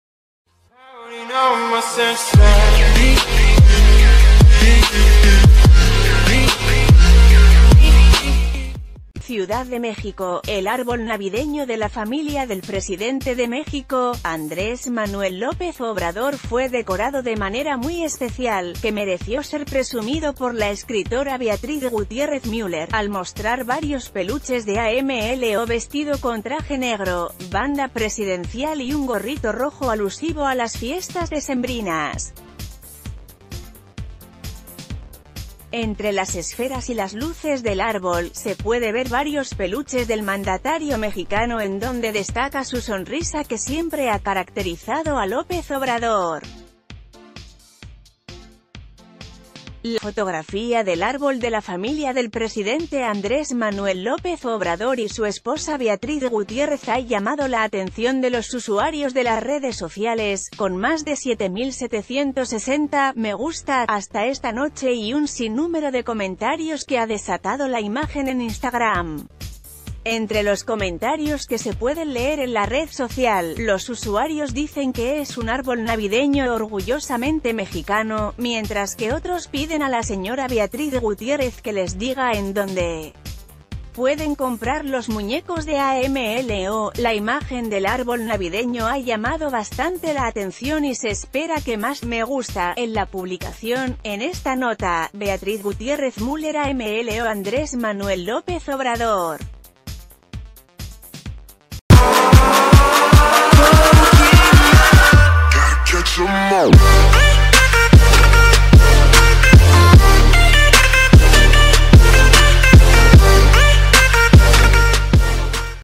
Beatriz Gutiérrez decora árbol navideño con peluches de AMLO
Ciudad de México.- El árbol navideño de la familia del presidente de México, Andrés Manuel López Obrador fue decorado de manera muy especial, que mereció ser presumido por la escritora Beatriz Gutiérrez Müller, al mostrar varios peluches de AMLO vestido con traje negro, banda presidencial y un gorri...